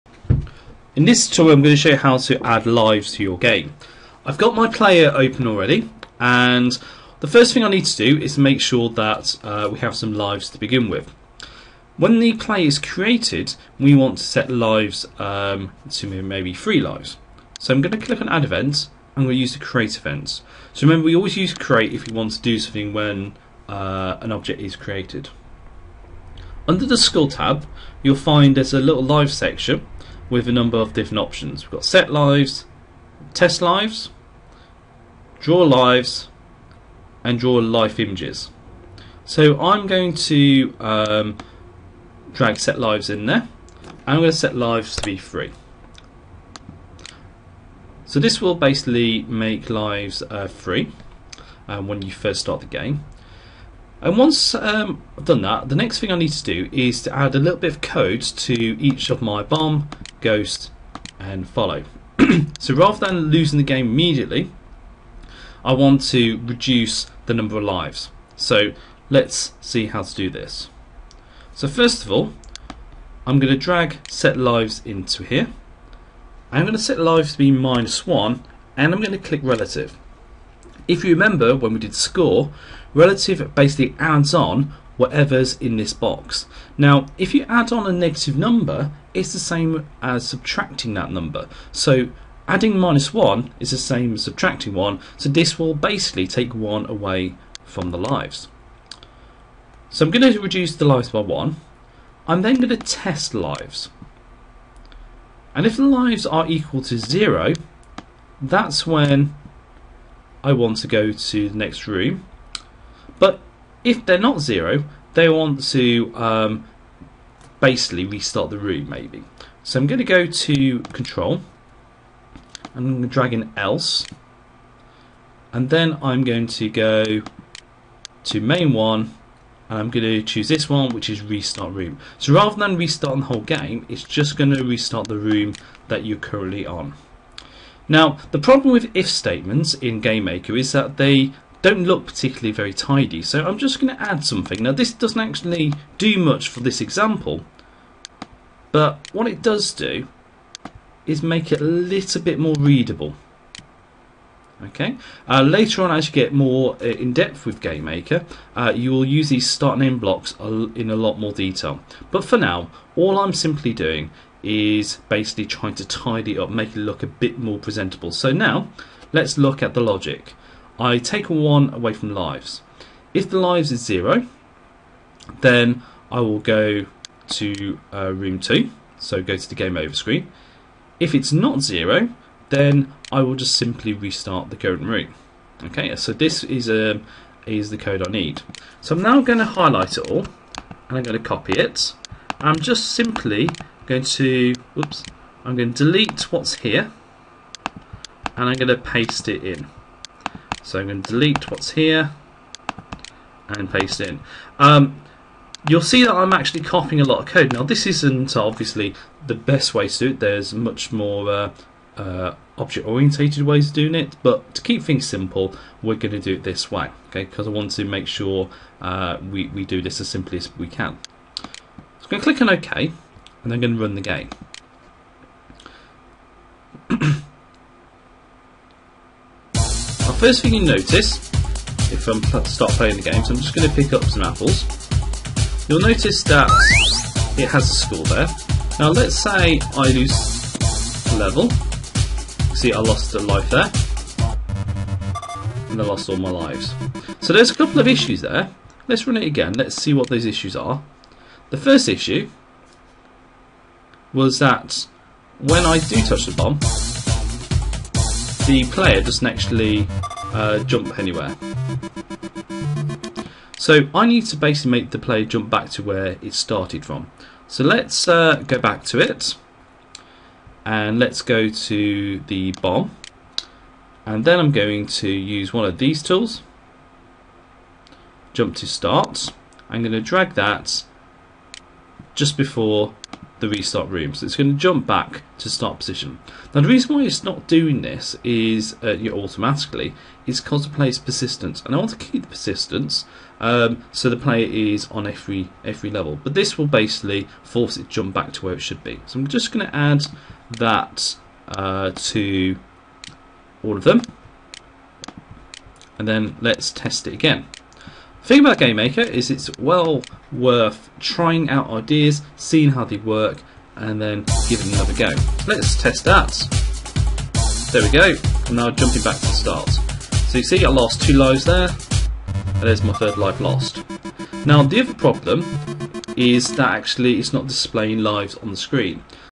Gamemaker - Adding lives
This tutorial will show you how to get your game to have lives. This is the first part of the tutorial with the second part showing you how to display lives on the screen.